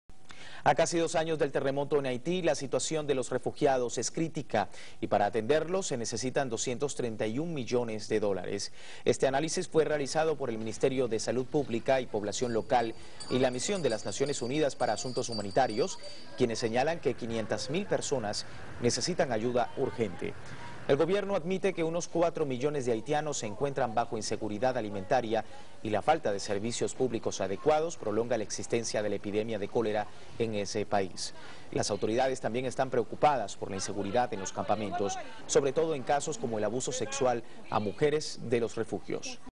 Haitian refugees in a critical situation
Almost two years after the earthquake in Haiti, the situation of the refugees remains critical and 231 millions of dollars are necessary to bring them attention according to a study carried out by the UN and local authorities.  These are saying that almost 500 thousands of people need assistance. teleSUR